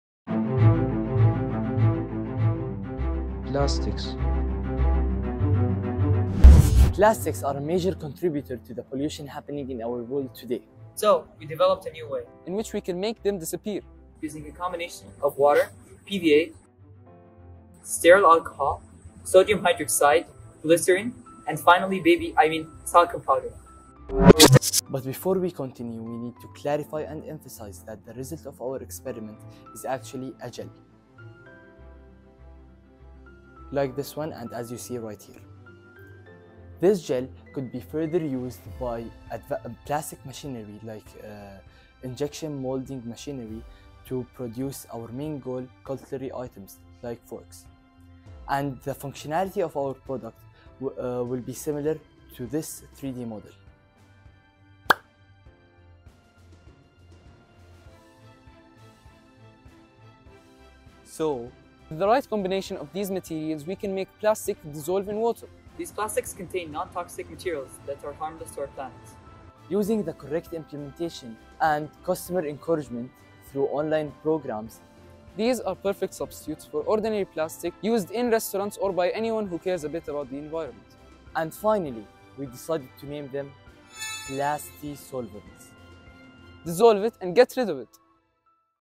Conrad Challenge: Innovation Stage Video -Plastisolvables Team-
This my team's innovation stage video for the Conrad Challenge 22-23.